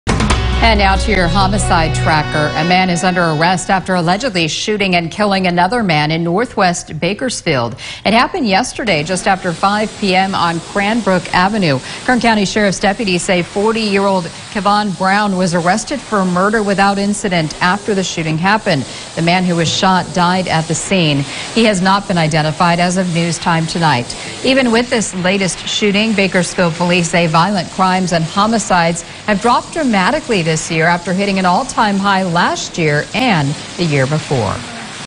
1 dead, 1 in custody after shooting in northwest Bakersfield
1 dead, 1 in custody after shooting in northwest Bakersfield